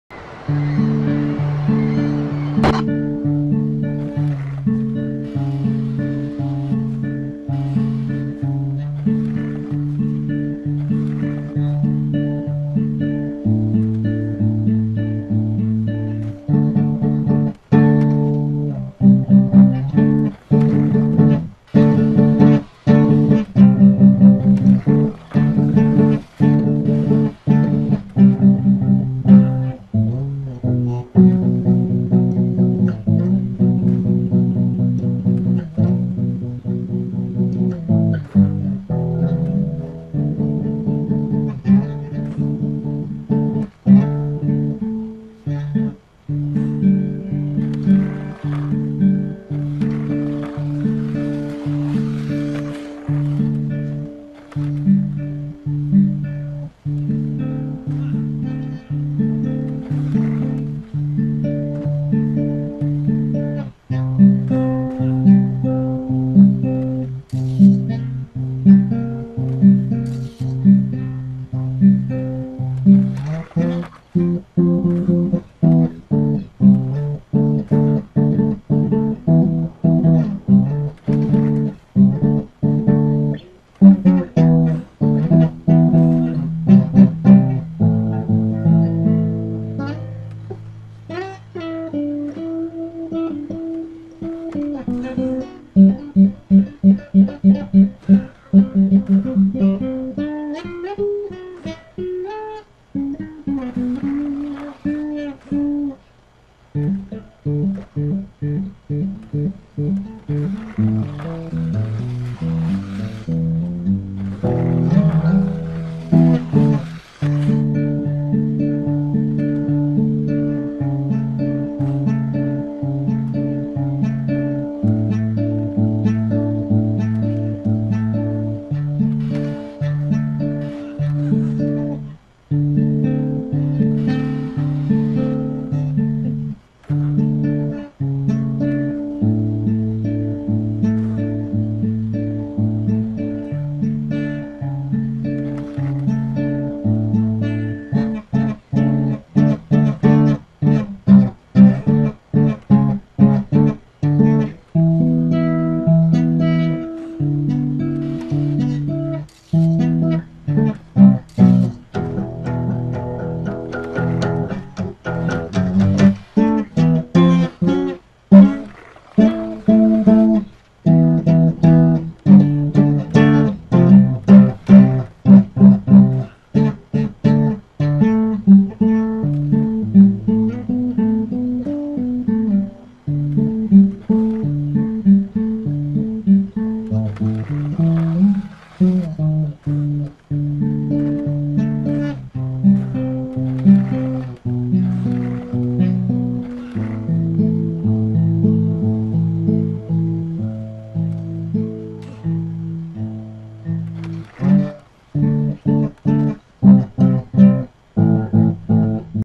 Venice beach with guitar
Tony Hawks Venice beach whilst the dark horse is playing guitar